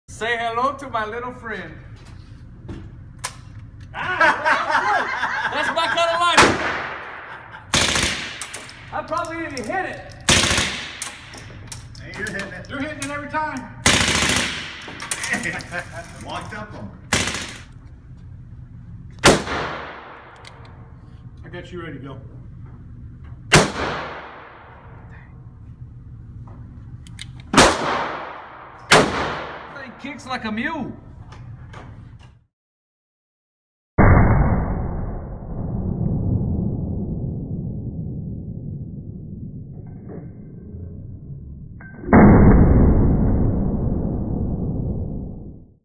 Bud and Broadway Go To Ultimate Defense
Ultimate Defense is in St. Peters, Mo.  We went there 3-16 and shot some awesome weapons.  Jerry is an accurate marksman and Bud its not.